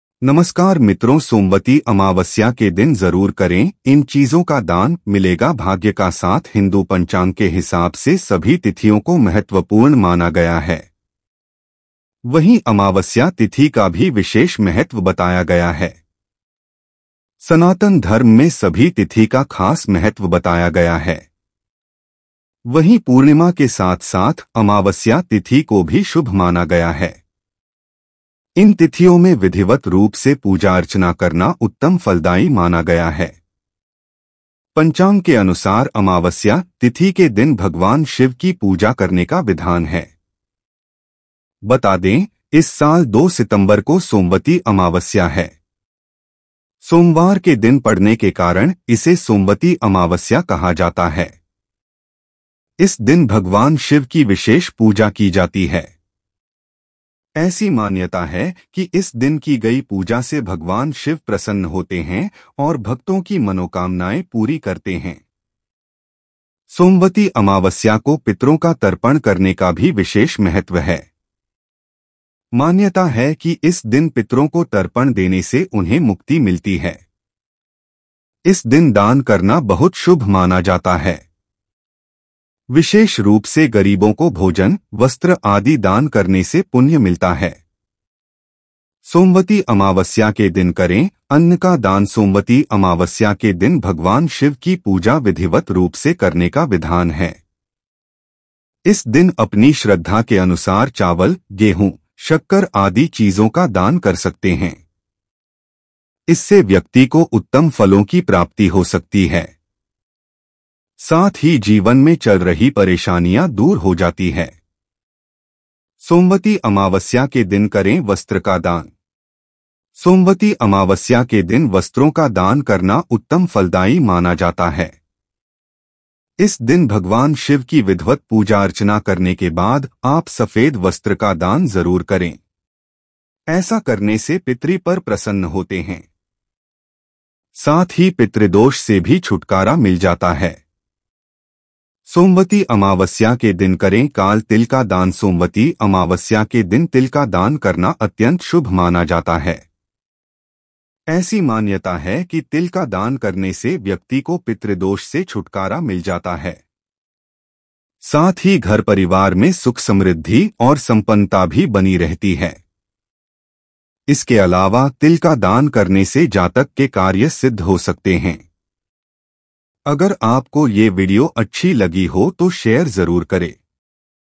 somwati amavasya 2024 सोमवती अमावस्या के दिन जरूर करें इन चीजों का दान, मिलेगा भाग्य का साथ।
Bhadrapad Somvati Amavasya 2024 Daan
Somwati amavasya somvati amavasya Puja vidhi, somvati amavasya ki Puja kaise karen, somvati amavasya vrat katha

सनातन धर्म में सभी तिथि का खास महत्व बताया गया है। वहीं पूर्णिमा के साथ-साथ अमावस्या तिथि को भी शुभ माना गया है। इन तिथियों में विधिवत रूप से पूजा-अर्चना करना उत्तम फलदायी माना गया है। पंचांग के अनुसार अमावस्या तिथि के दिन भगवान शिव की पूजा करने का विधान है।